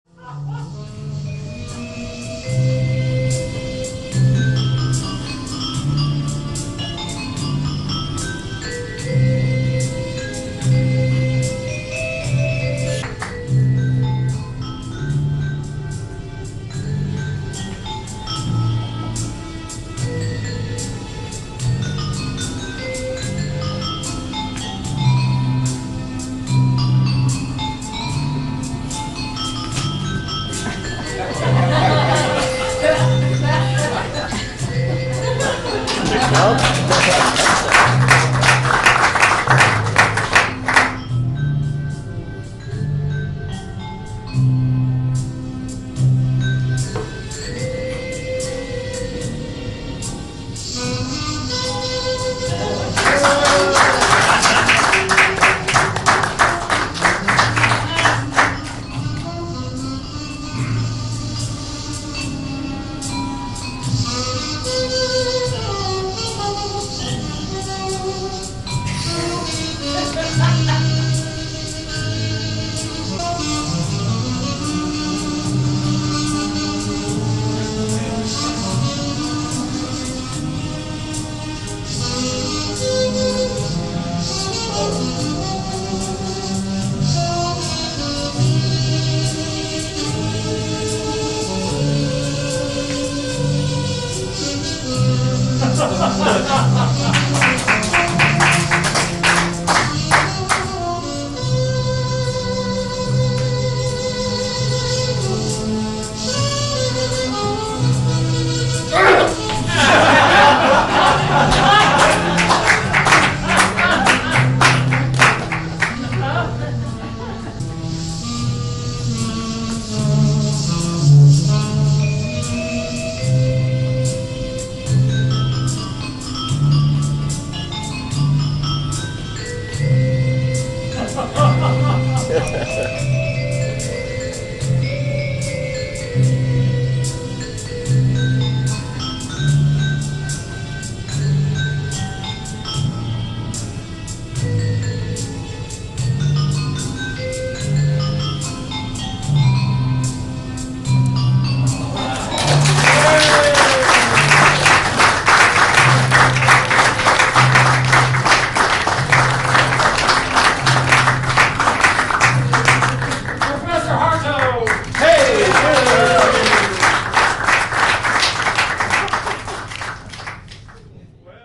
"Prof. Harto" Comedy Magic
Here are few clips from my magic comedy act, Prof. Harto - played by Steve Hart